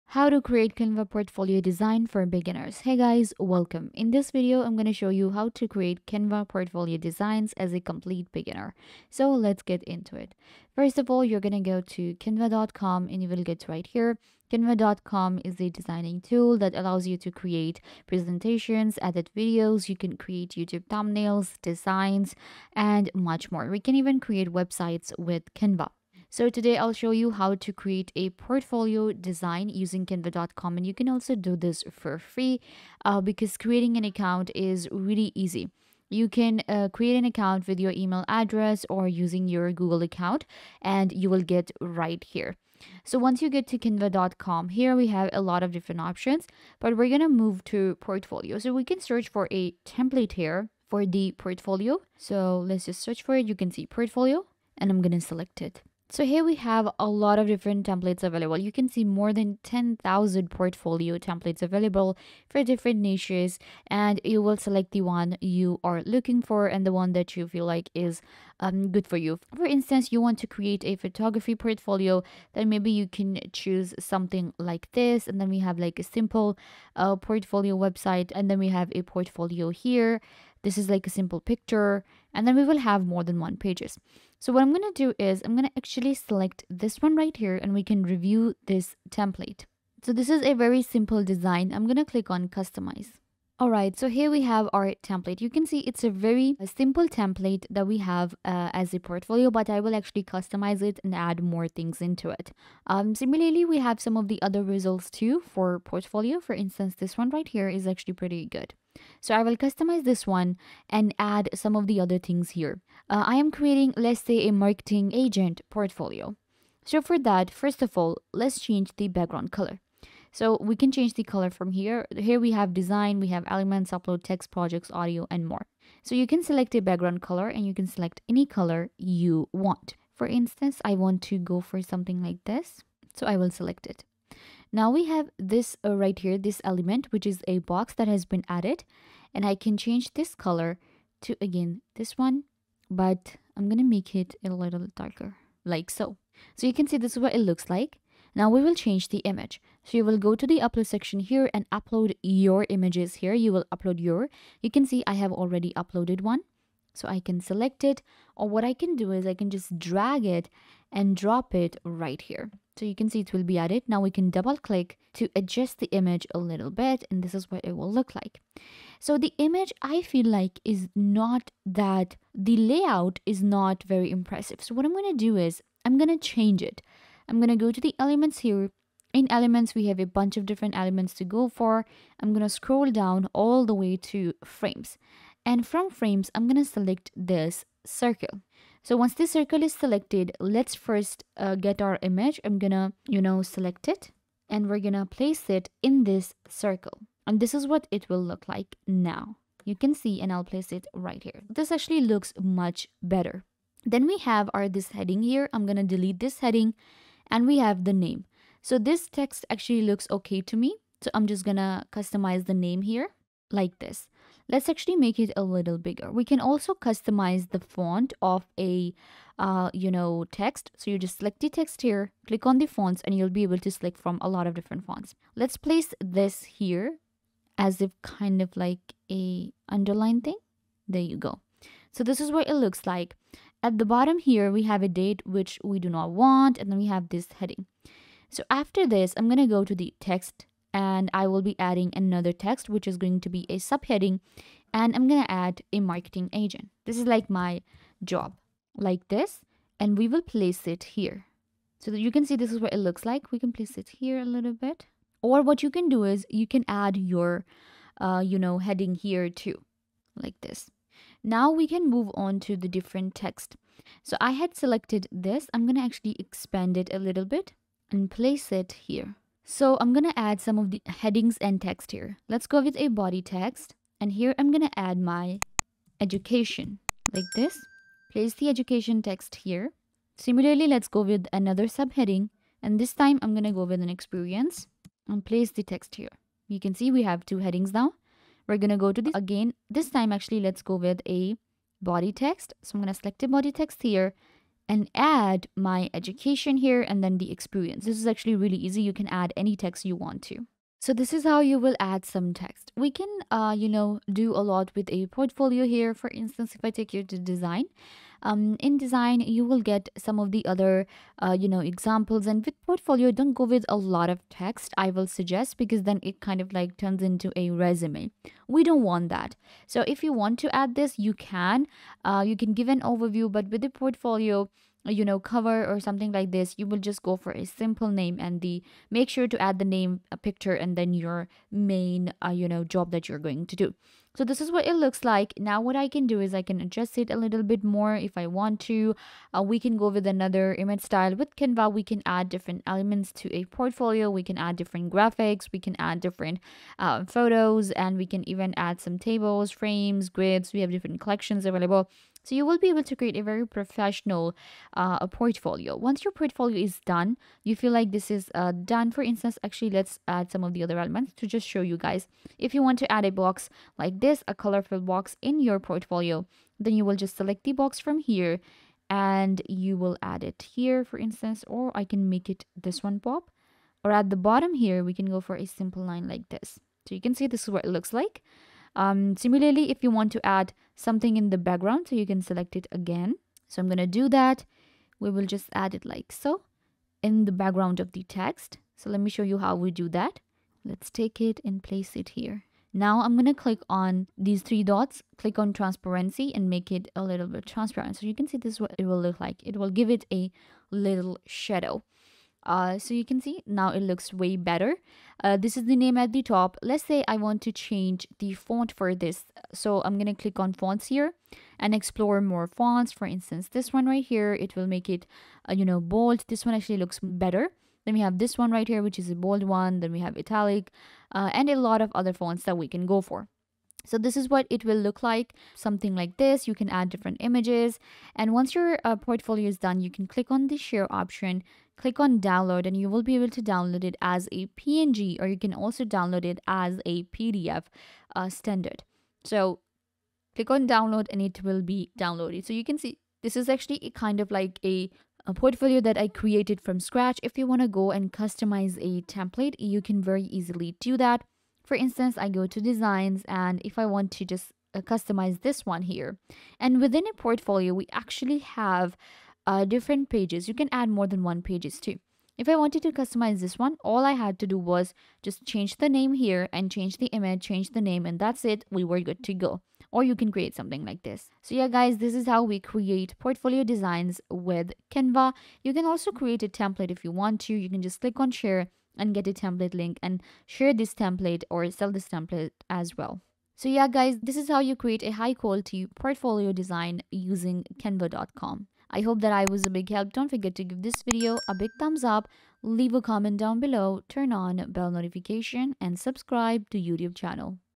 How To Create Canva Portfolio Design (For Beginners)
In this video I show you How To Create Canva Portfolio Design. It is really easy to do and learn to do it in just a few minutes by following this super helpful tutorial to get a better understanding of everything.